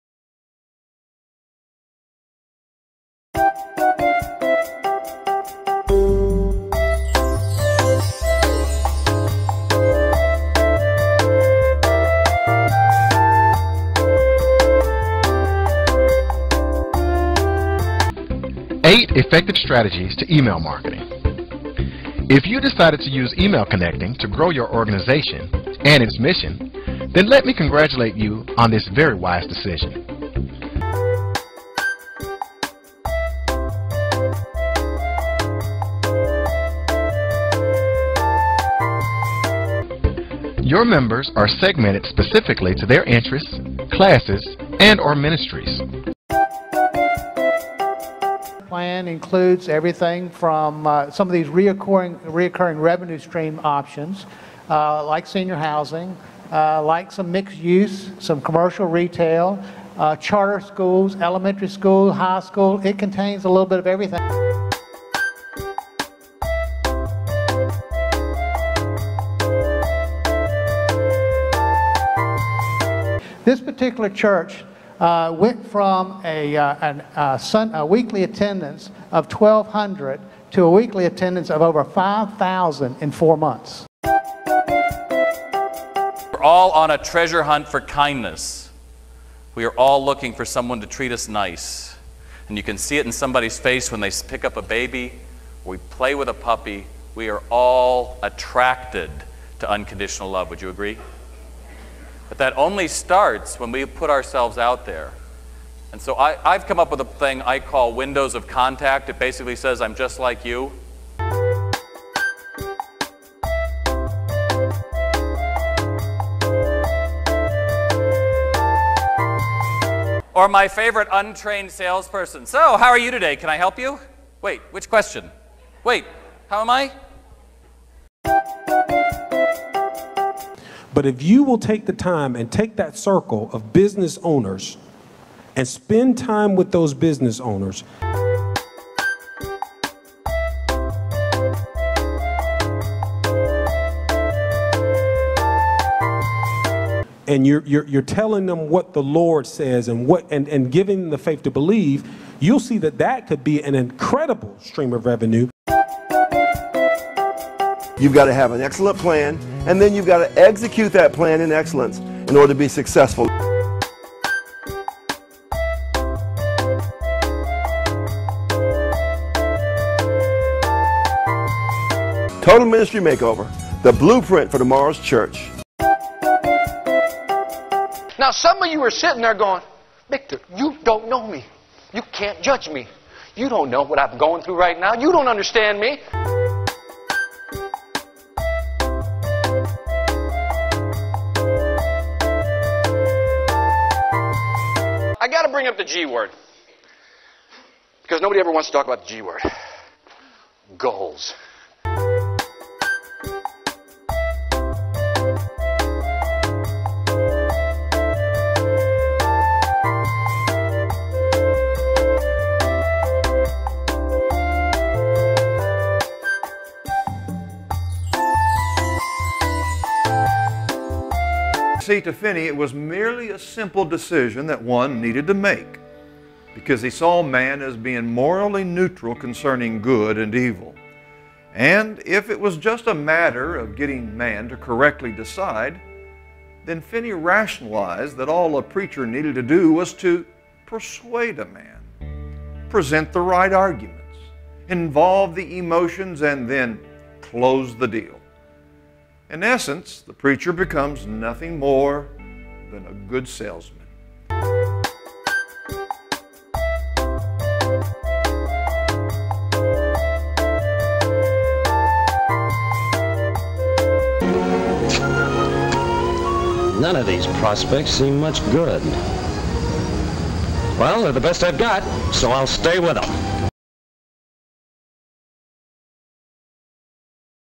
Quiz: Sales OR Church Seminar
Here's a little quiz. Are the video clips from a sales seminar OR a church growth/motivation seminar?